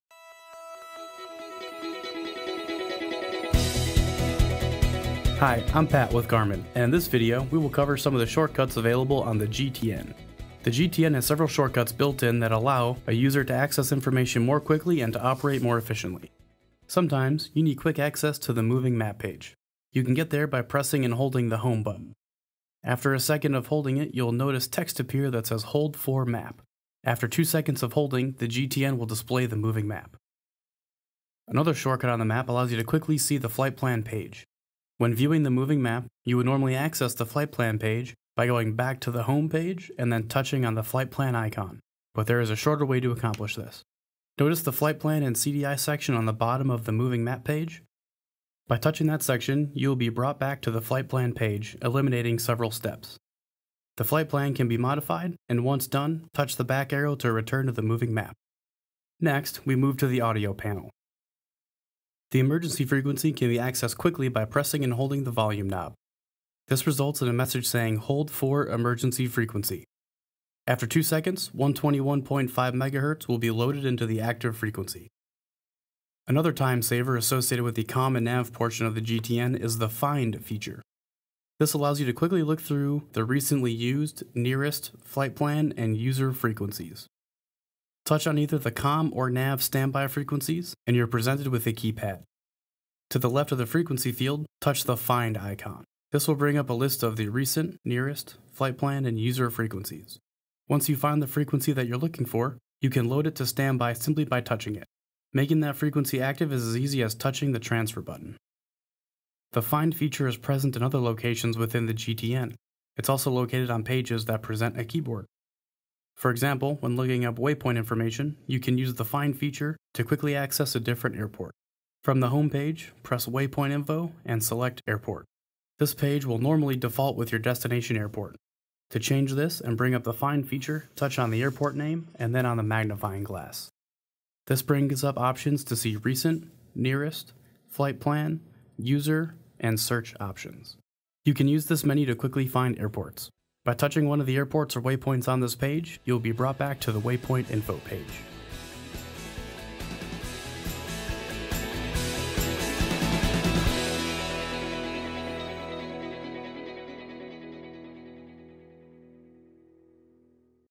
Garmin GTN™ Series: Saving You Time - Shortcuts
Learn how to access information more quickly and efficiently on your GTN GPS/NAV/COM/MFD solution with these shortcuts.